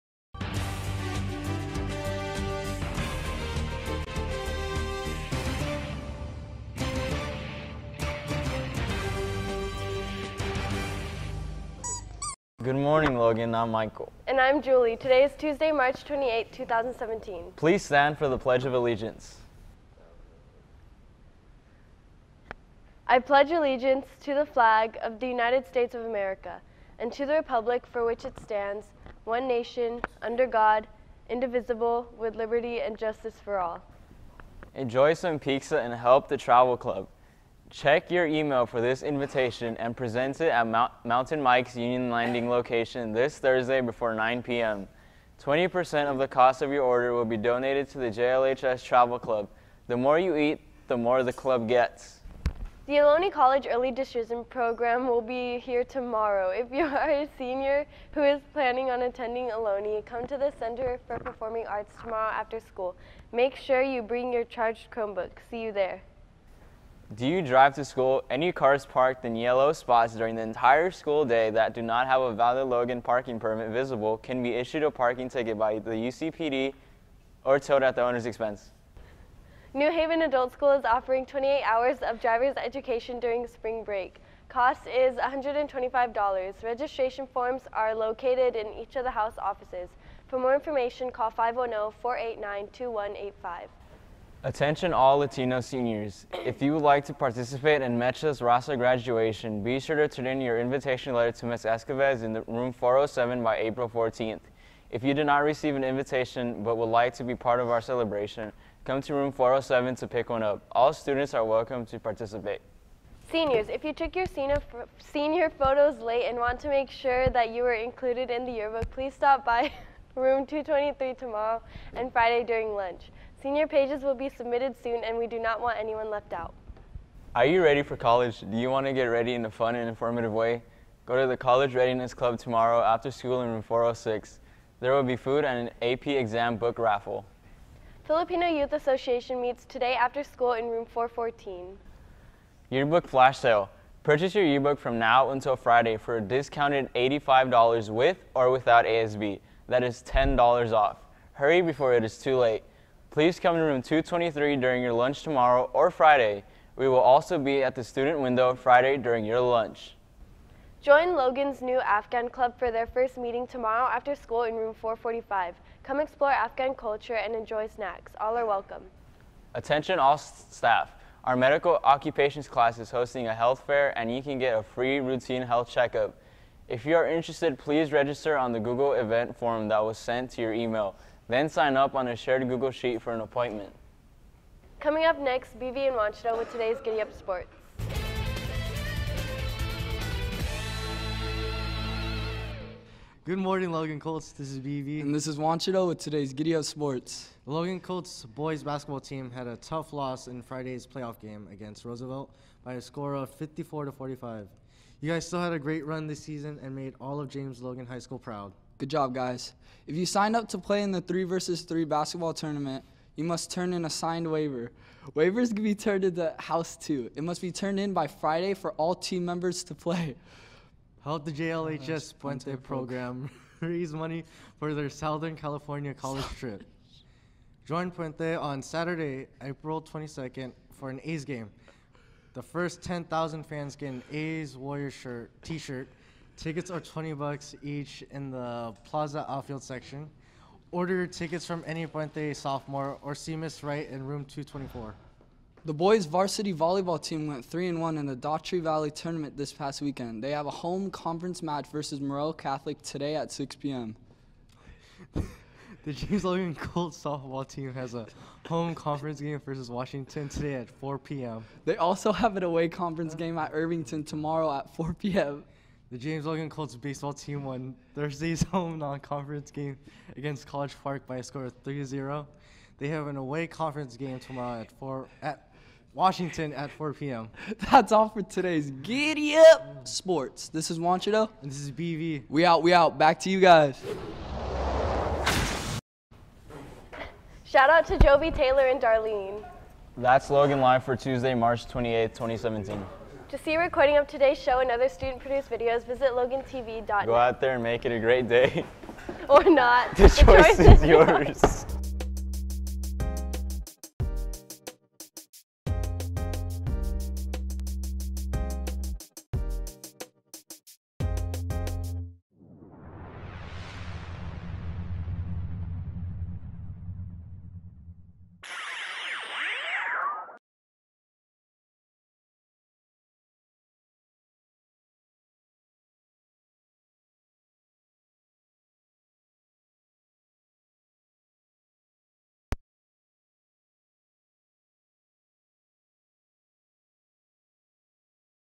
Logan Live 3/28/17
Logan Live is the daily student-produced newscast from James Logan High School in Union City, California. This is the recording of the 3/28/17 show. Logan Live is broadcast most school days at the beginning of second period: 9:05AM Wednesdays, 9:20AM other weekdays.